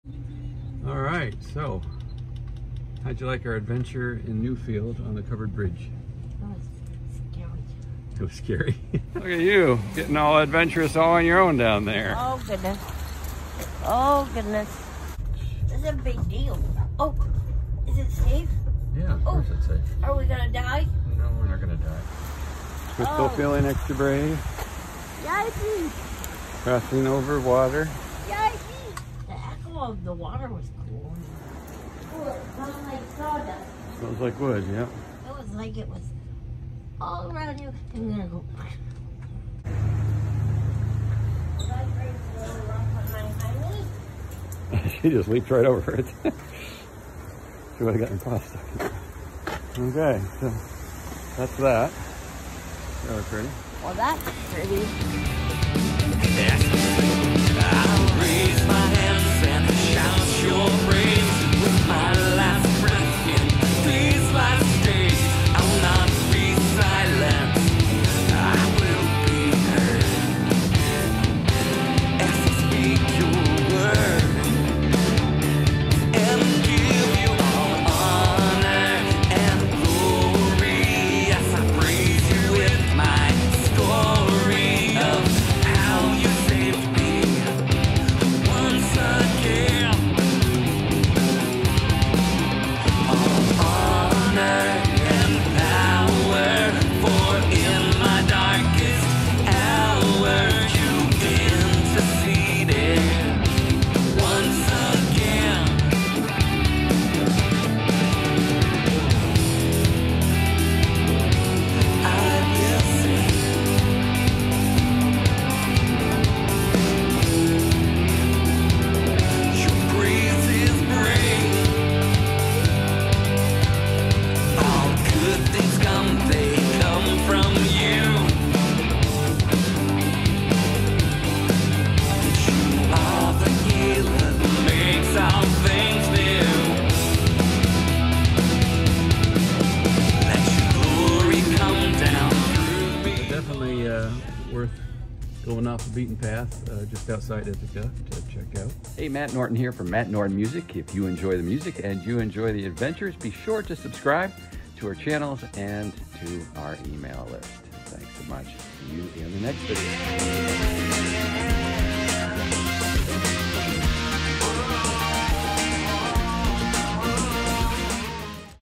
Matt Norton wheelchair Accessible Adventures - Newfield NY Covered Bridge
Let the outdoor adventures of spring 2021 begin.

One of the last remaining original covered bridges in the US similar to the one featured in The Bridges of Madison County.

Matt Norton, Crystal Lane Brehm and their Labrador mix puppy ￼Eva embark on another adventure one day shy of their secound covid vaccination￼.

Coming to the end of a long stressful week with early mornings of Crystal meeting with politicians to advocate for the disabled via zoom we enjoyed a little side trip on the way to visit with my parents.

Watch for upcoming live in person concerts in July and August!